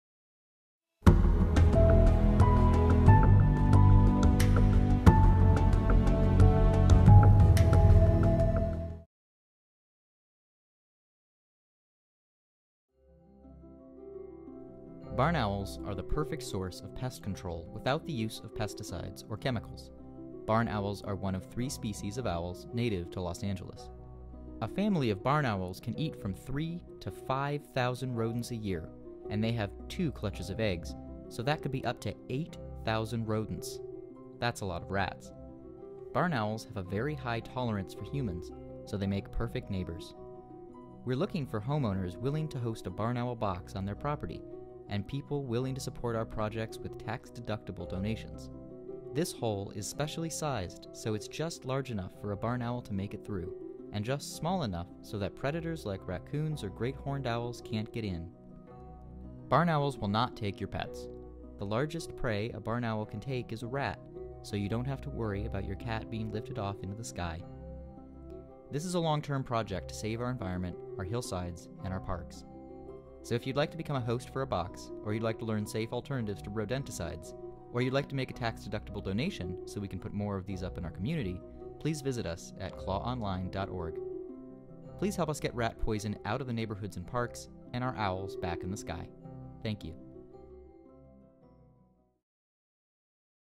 CLAW barn owl nesting box program
The Barn Owl Nesting Box Program is part of CLAW's work to end the use of rodent poison (rodenticide) in Los Angeles and beyond. Rodenticide is extremely dangerous to the health of our urban/wildlands interface communities -- and the precious wildlife who share them with us.

to learn more about alternatives and how you can get involved.

Just one barn owl can consume more than 3,000 rodents a year.  Barn owls are already naturally helping to control rodent populations in vineyards, farms and ranches, in California and across the world. Luckily, barn owls have a high tolerance for humans and often choose to nest in proximity to us, making perfect neighbors even in an urban setting--such as the barn owl populations already living right here in Los Angeles.